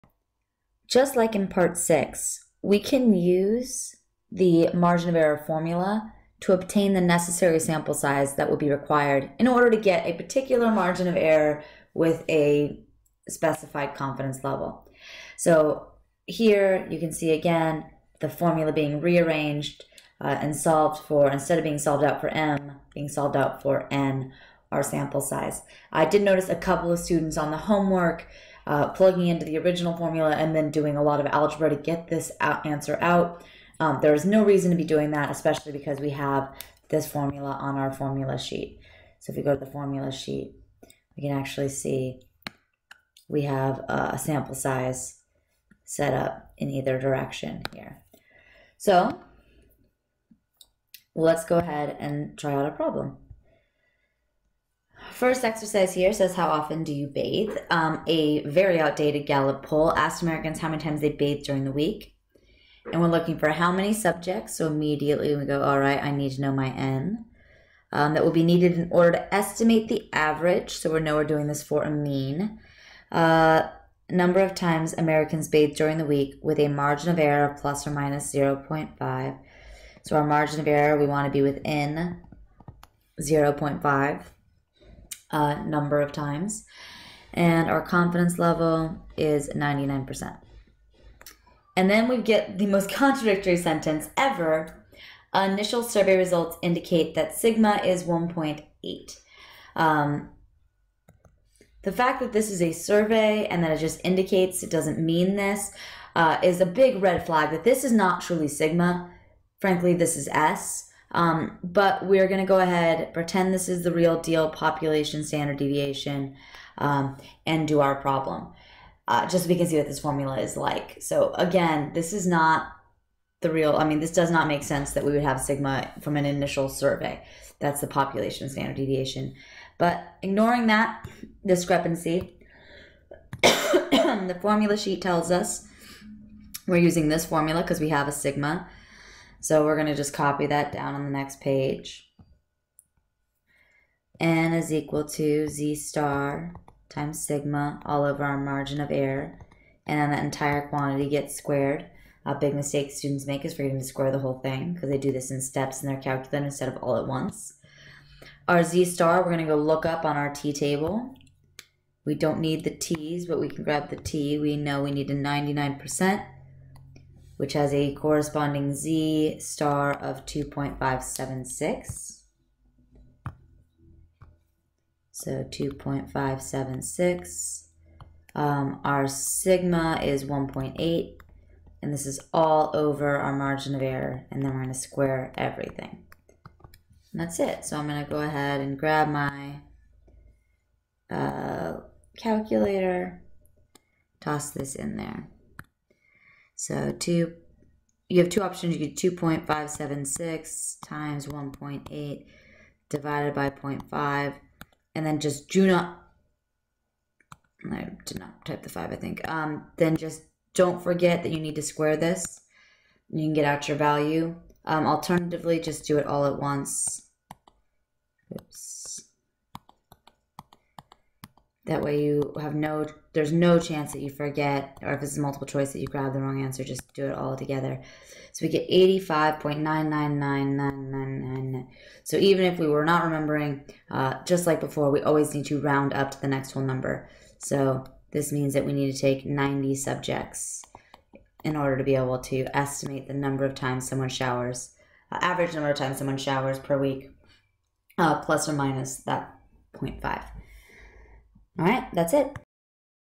Math 119 Video Lecture - 7.3 Sample Size Calculations for Means
Includes Exercise 7.3.1 and a moment of numerical insanity.